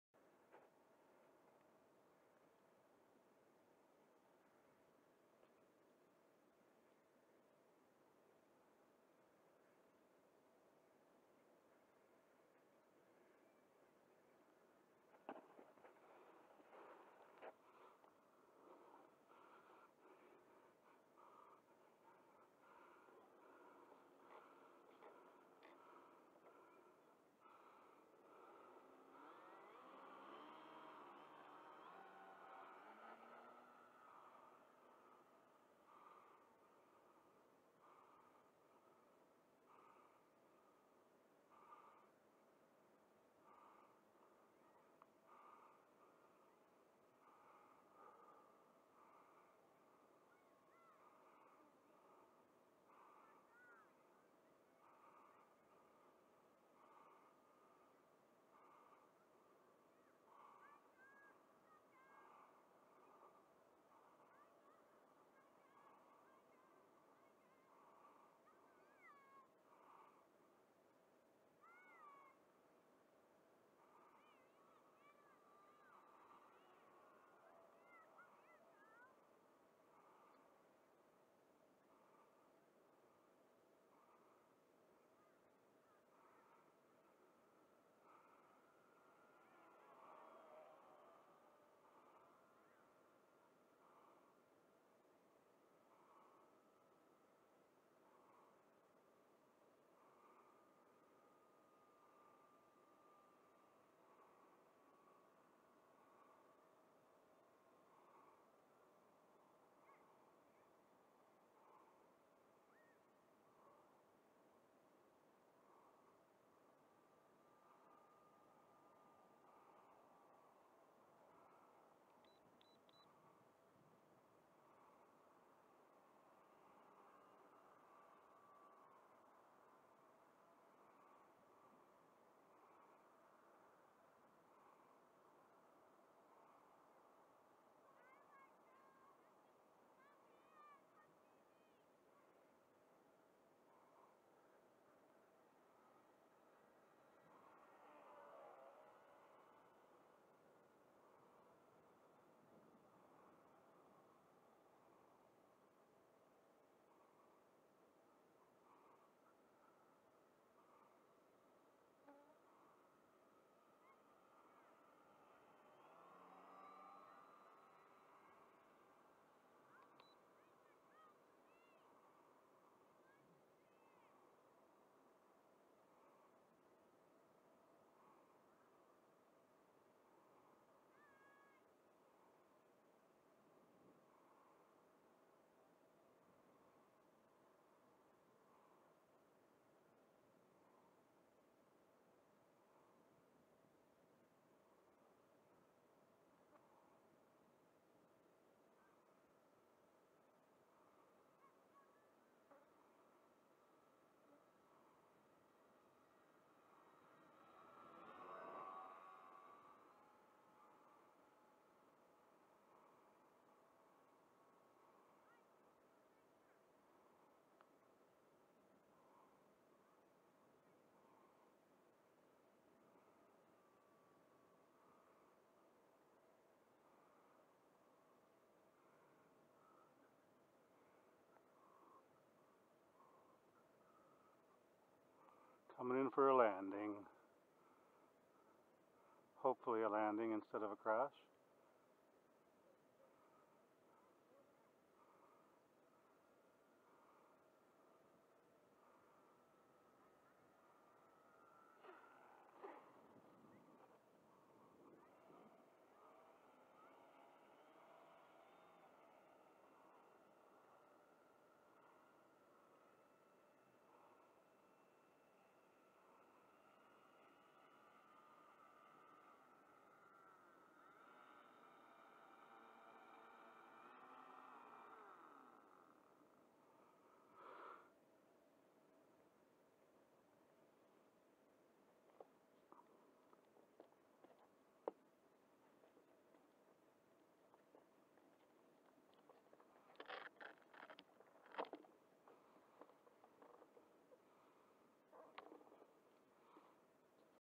Flybeam Night Flight
Enjoying flight after dark with my rebuilt Hobbyking Flybeam on Sept. 4th 2016.